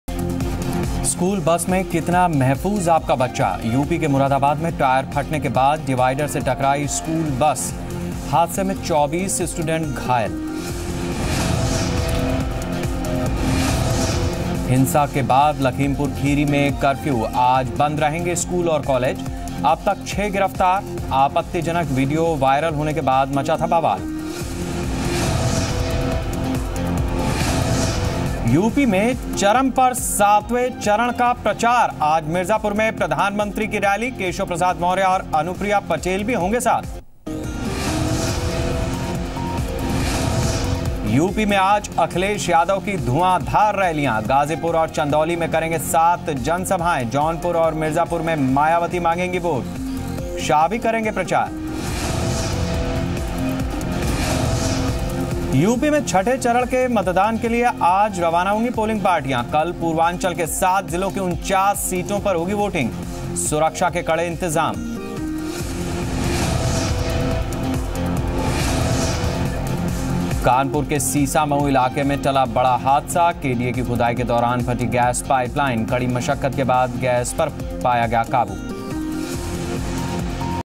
School bus hit divider leaves 24 students injured
School bus hit divider leaves 24 students injured. Watch this special segment and get to know more here.